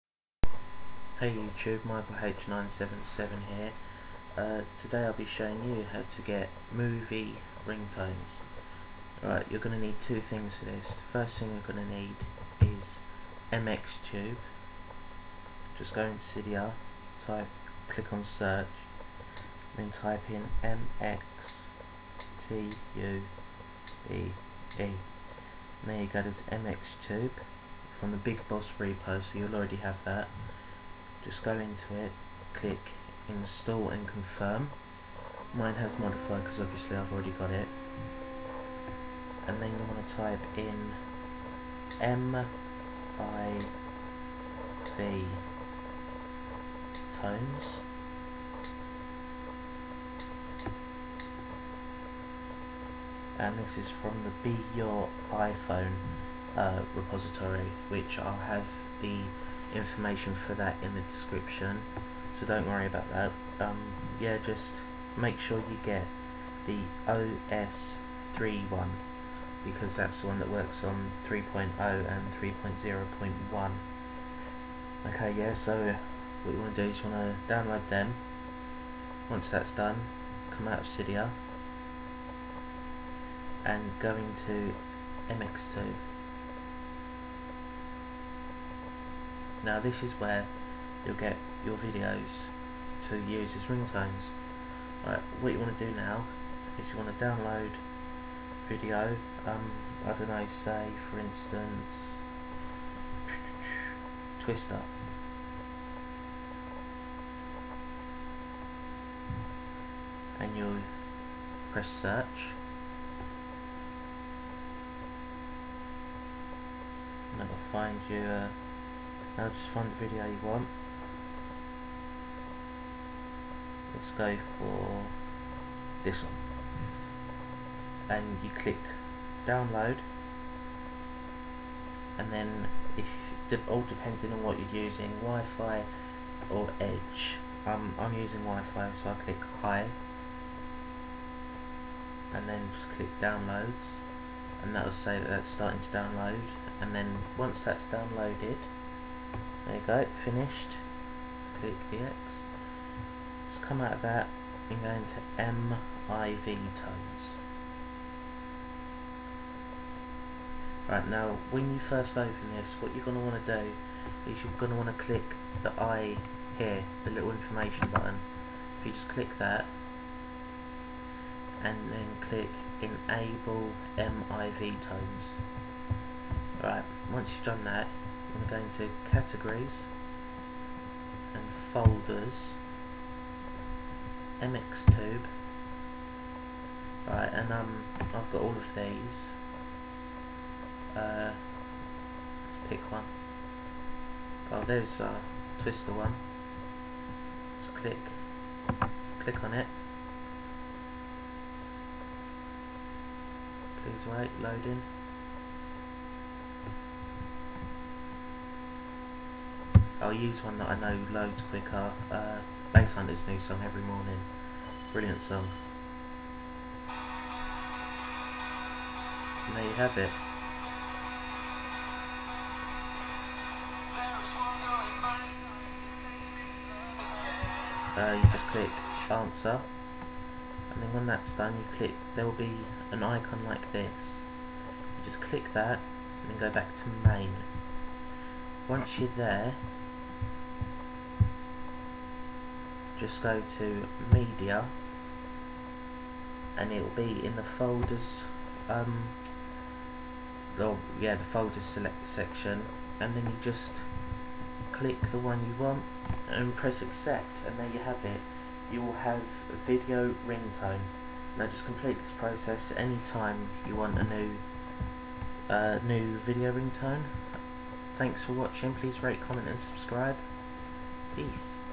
Video ringtones
hey everybody this is how to get video ringtones

1. open cydia and let it load

2. go to manage, sources, edit, add

4. go to search and search mivtones per os 3.x when found press install and confirm

5. when that is done go to search again and type in mxtube and install and confirm

6. now exit cydia and open mxtube and go to search and search for the video of your choice

7. when found press it and press download and choose high (wifi)

8. press downloads and wait for it to finish when finished close out of mxtube

9. now open mivtones on the open screen press the i on the top right hand corner

10. now press enable video, make sure you NEVER press logout

11. now press categories, folders, mxtube now find your video

12. when found press the little plus sign (in blue) next to the video

13. now go back to main (you may need to press the little i again) press media

14. now press the video and it will start to play press awnser and you will have a video ringtone sorry you cant see mine in the video as im useing VNC viewer